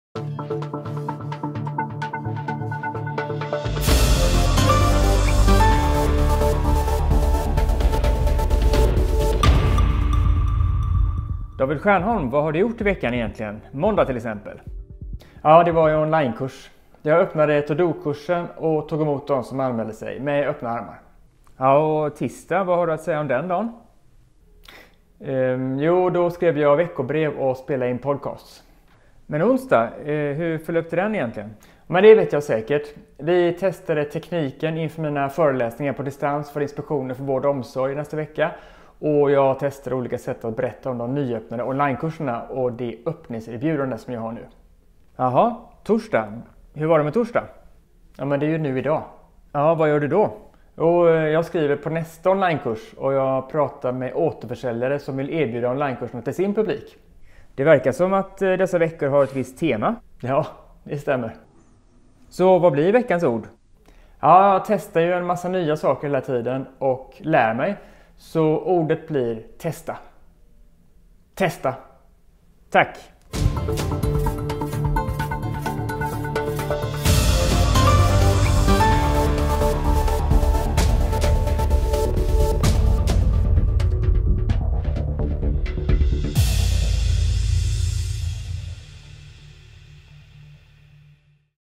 Vecka 20, 2020: Testa
Jag blev kritiskt granskad och utfrågad på jobbet den här veckan, vilket verkligen inte var vad jag hade väntat mig.

Nåväl. Veckans ord blev "testa".